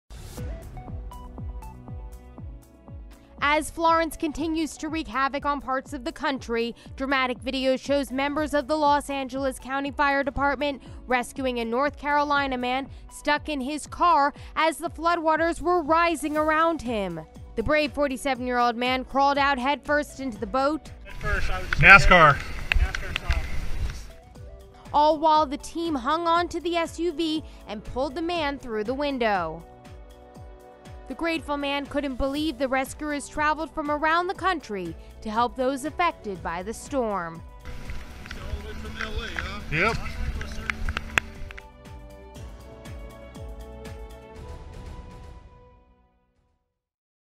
Brave Firefighters Save Man Trapped In Car During Florence Flooding
As Florence continue to wreak havoc on parts of the country, dramatic video shows members of the Los Angeles County Fire Department rescuing a North Carolina man stuck in his car as the floodwaters were rising around him.